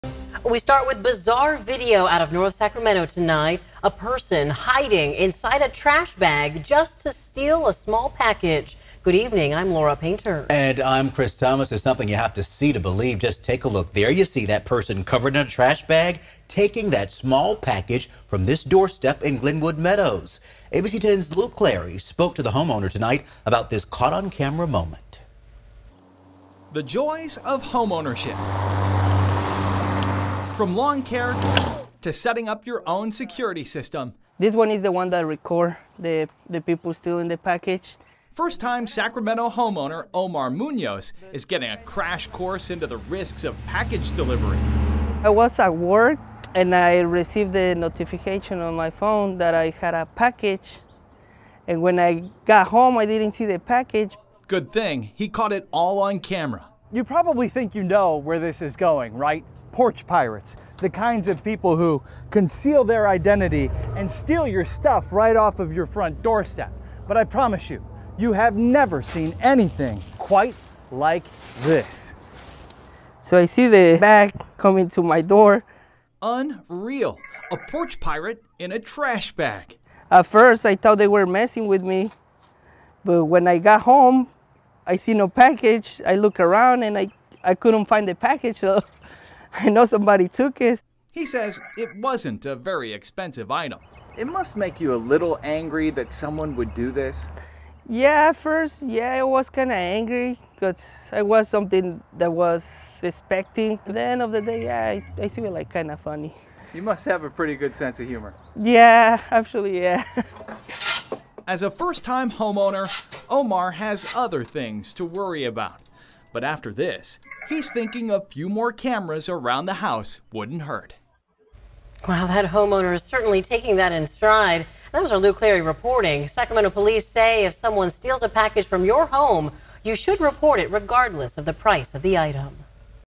Video: Porch pirate uses trash bag for cover while stealing package in Sacramento
First-time Sacramento homeowner Omar Munoz is getting a crash course on the risks of package delivery.